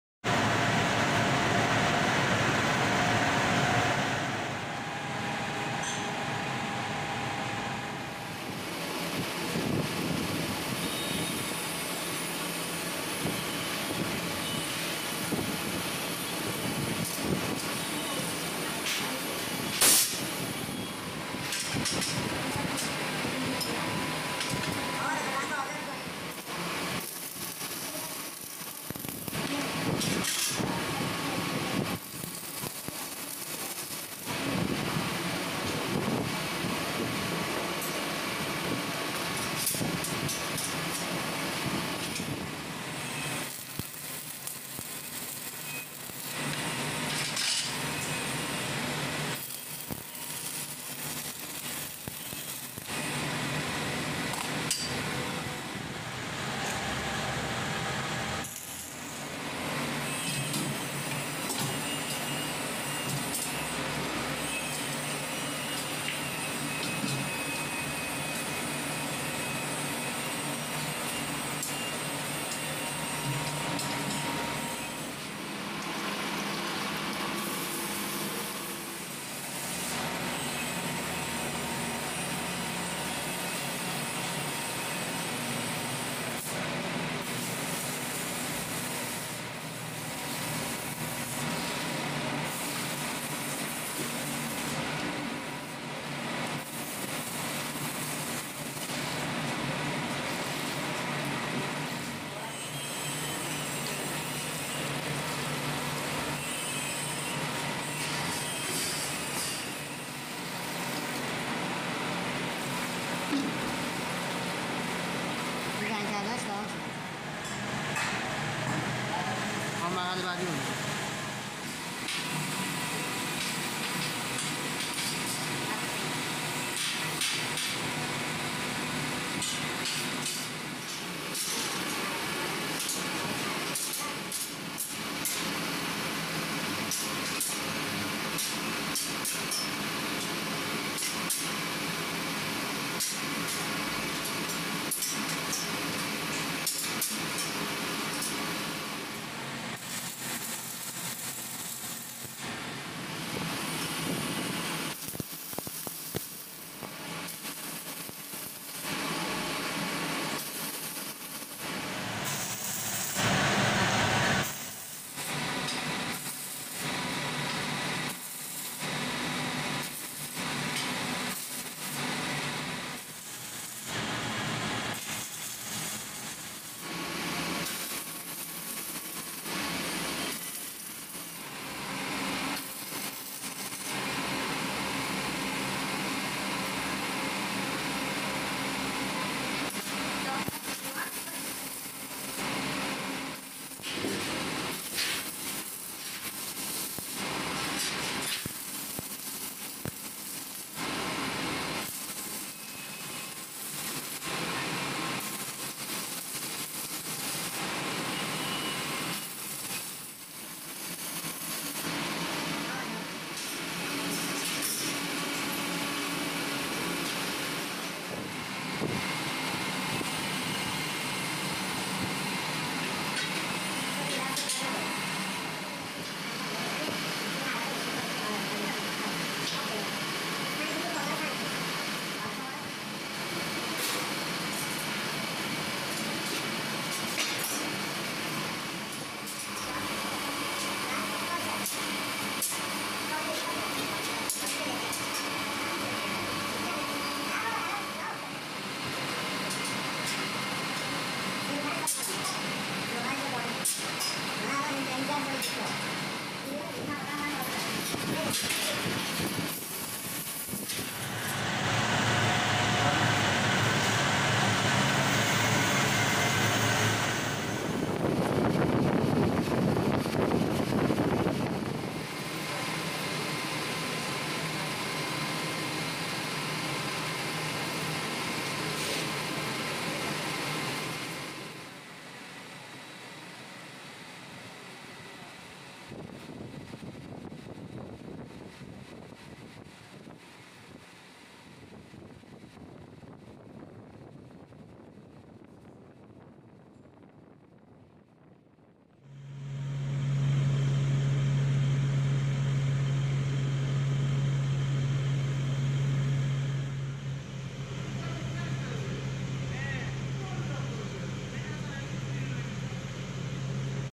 Making Metal Fence / How Is Made
In this video, you will see how to make a metal fence. These fences are usually used on the exterior walls of houses or yard walls.
I hope you enjoy watching this video.